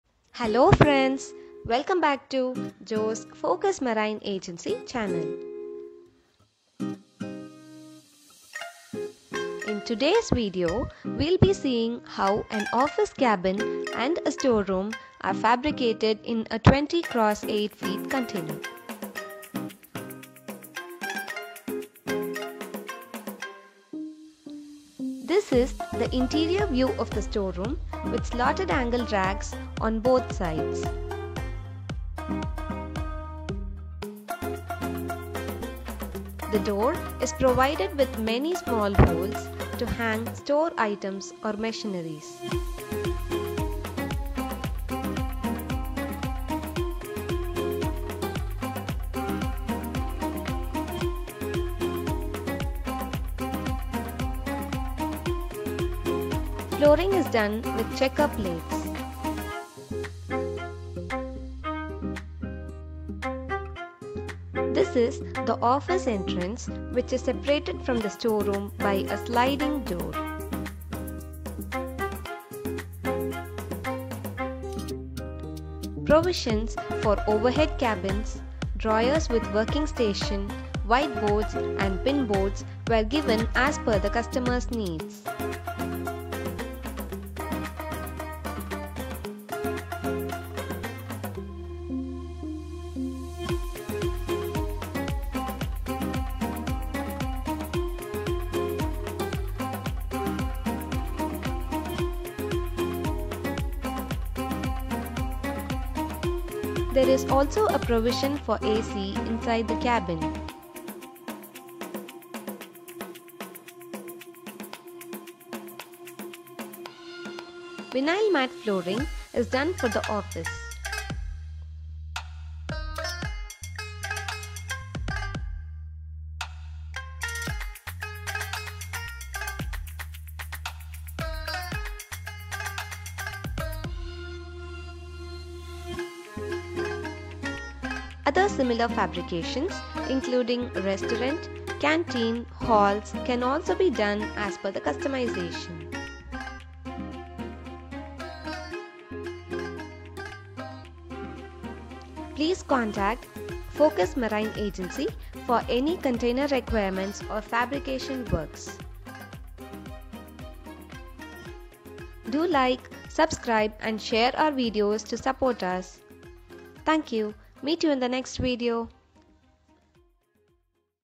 Cheap Office and Store Space in 1 Container - Level 19 E
Hi Friends,
                   Today we are going to show how the shipping container can be used both as storage place and as office space... This is multipurpose container and can be transported to any location as you all like... this is done for a MNC company...

For any kind of shipping  requirements,

Focus Mariine agencies
Chennai